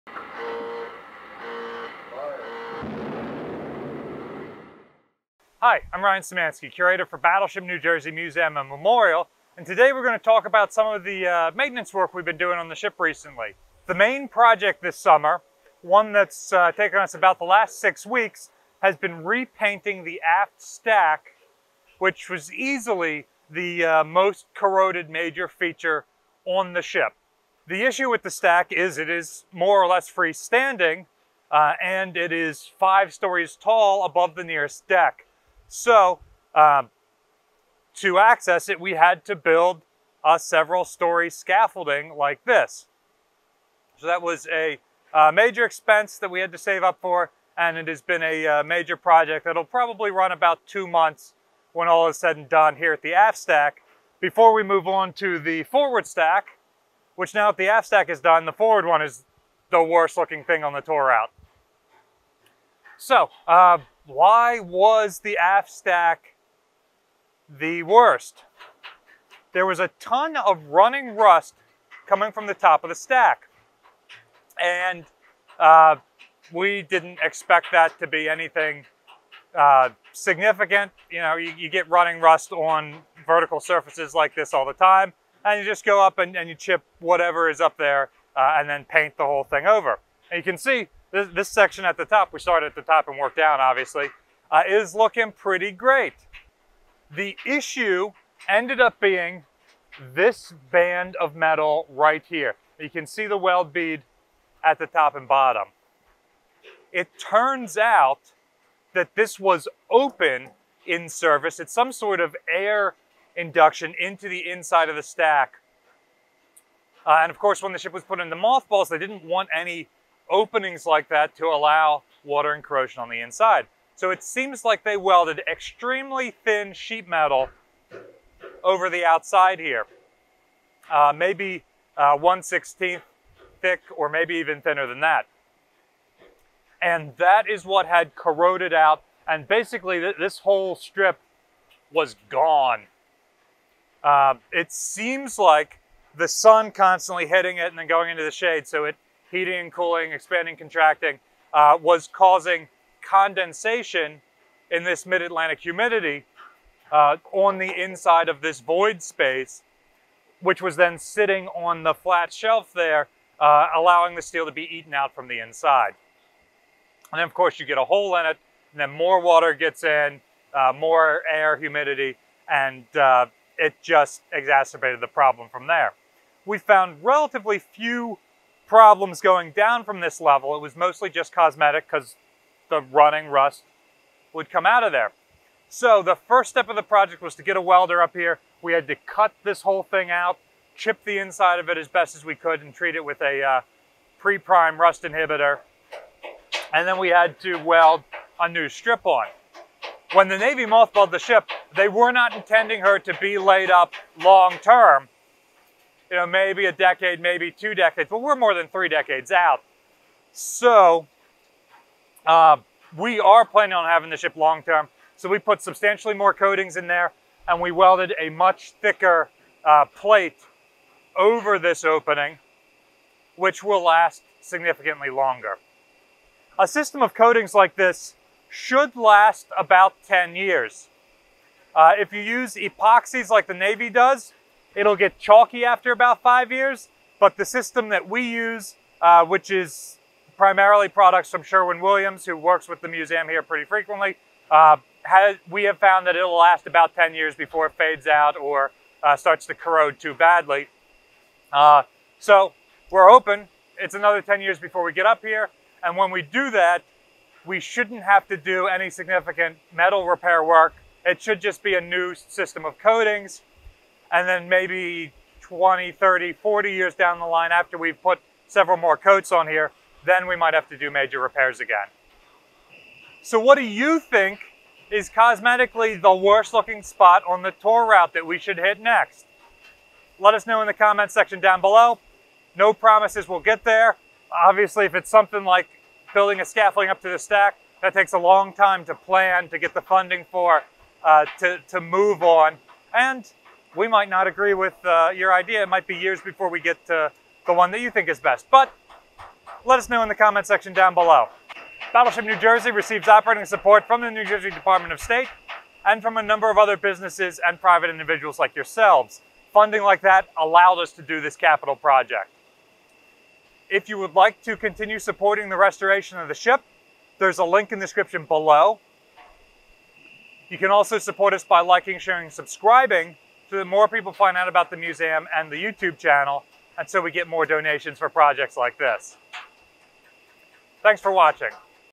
Restoring Battleship NJ's Smokestacks
In this episode we're talking about a major restoration project at the museum being done in summer 2022. Special thanks to Camden County for their support of this project.